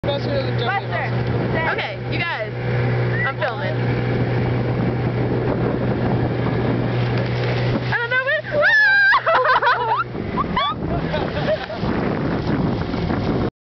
Cory joey boat jump
Cory and Joey Jumping off boat at the same time